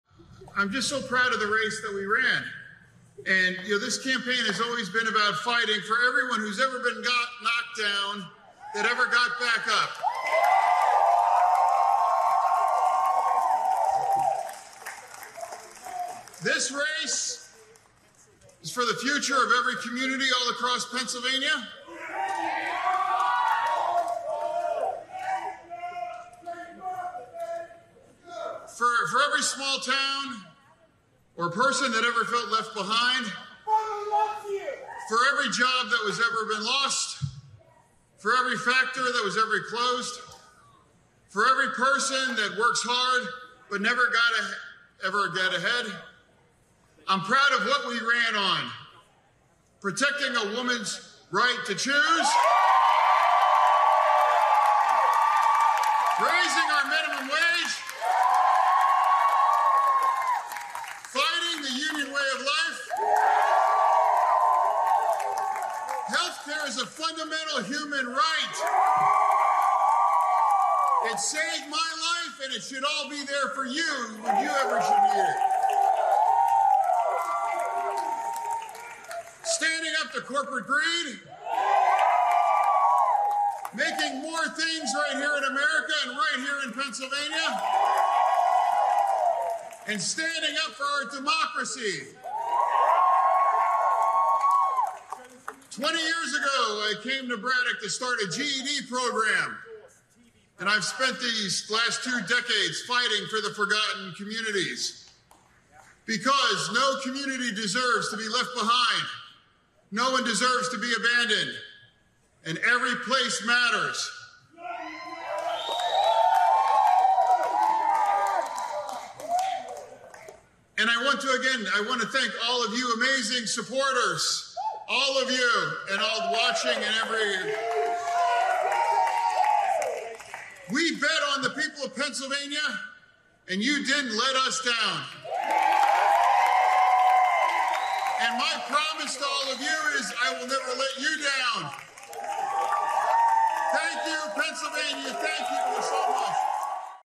John Fetterman: I’ll Be the Next Senator From Pennsylvania
Pennsylvania Democrat John Fetterman speaks to supporters after defeating Republican candidate Mehmet Oz in one of the country’s hardest fought Senate contests.